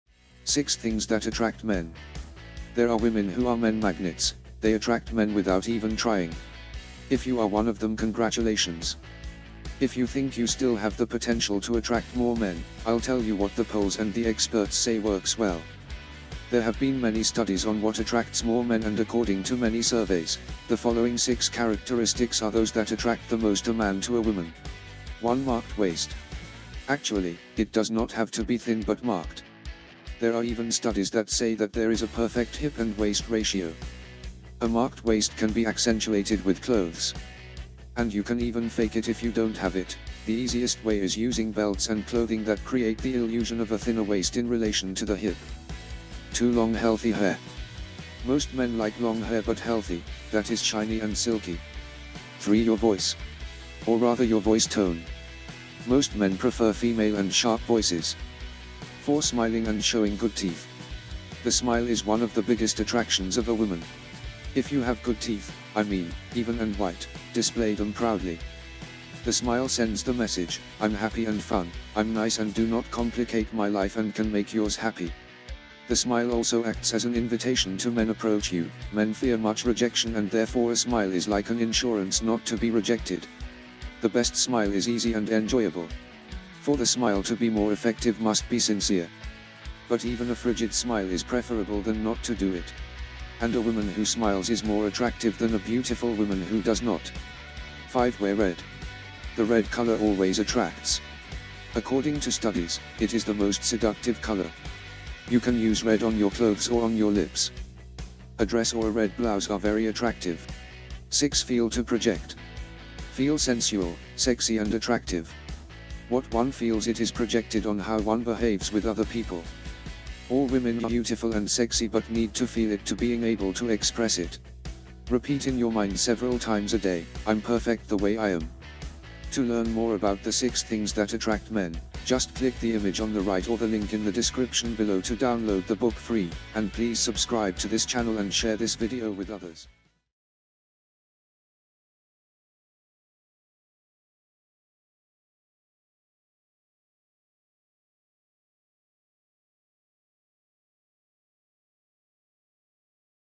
6 Things That Attract Men
To learn more about 6 Things That Attract Men, DOWNLOAD any of these 3 FREE EBOOKS, by following these links:

There are women who are men magnets, they attract men without even trying. If you are one of them congratulations. If you think you still have the potential to attract more men, I'll tell you what the polls and the experts say works well.

There have been many studies on what attracts more men and according to many surveys, the following 6 characteristics are those that attract the most a man to a woman.

1. Marked waist.

Actually, it does not have to be thin but marked. There are even studies that say that there is a perfect hip and waist ratio. A marked waist can be accentuated with clothes. And you can even fake it if you don't have it, the easiest way is using belts and clothing that create the illusion of a thinner waist in relation to the hip.

2. Long healthy hair.

Most men like long hair but healthy, that is shiny and silky.

3. Your Voice.

Or rather your voice tone. Most men prefer female and sharp voices.

4. Smiling and showing good teeth.

The smile is one of the biggest attractions of a woman. If you have good teeth, I mean, even and white, display them proudly. The smile sends the message; I'm happy and fun, I'm nice and do not complicate my life and can make yours happy. The smile also acts as an invitation to men approach you, men fear much rejection and therefore a smile is like an insurance not to be rejected.

The best smile is easy and enjoyable. For the smile to be more effective must be sincere. But even a frigid smile is preferable than not to do it. And a woman who smiles is more attractive than a beautiful woman who does not.

5. Wear red.

The red color always attracts. According to studies, it is the most seductive color. You can use red on your clothes or on your lips. A dress or a red blouse are very attractive.

6. Feel to project.

Feel sensual, sexy and attractive. What one feels it is projected on how one behaves with other people. All women are beautiful and sexy but need to feel it to being able to express it. Repeat in your mind several times a day; "I'm perfect the way I am".